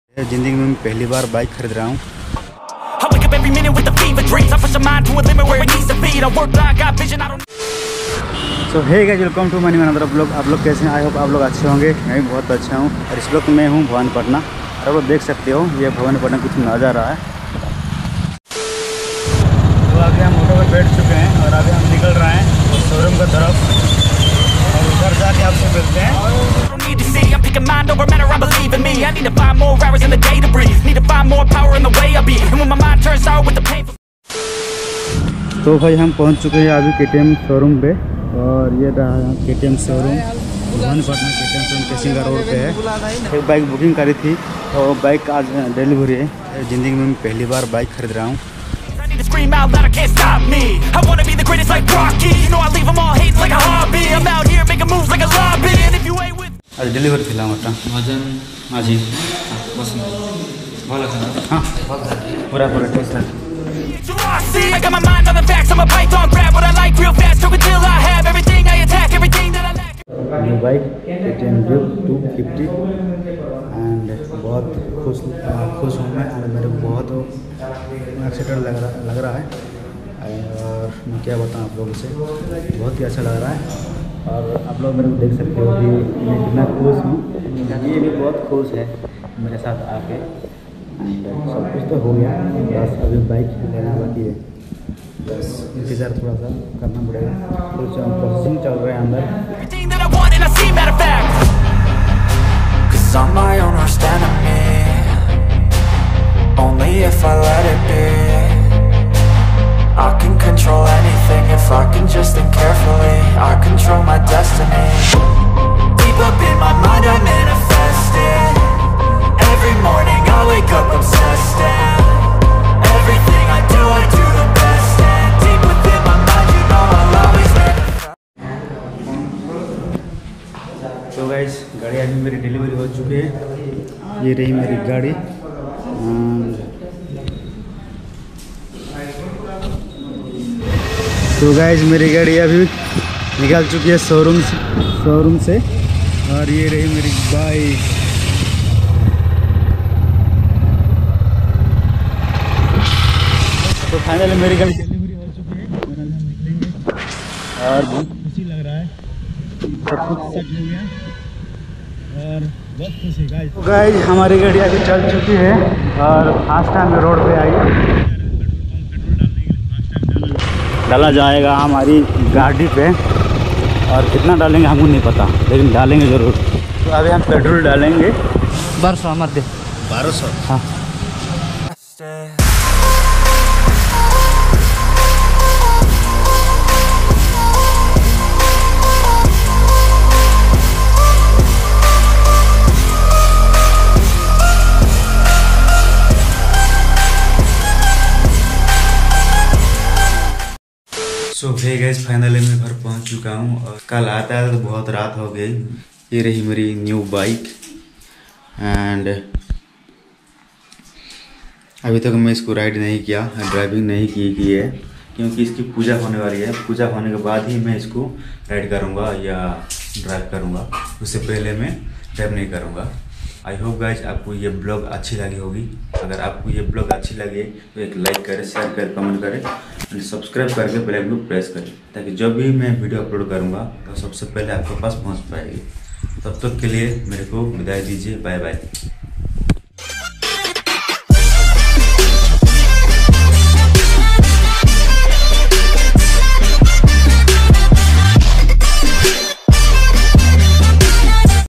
Finally ❤️❤️Dream Comes True | Taking Delivery Of My New Super Bike | Bhajan Majhi Vlogs
Hay Guys Finally Aji Meri Life ka Peheli dream bike apni kamaise lehili aj mujhe bahat khus lag raha hai or excited

If you like this vlog then Please hit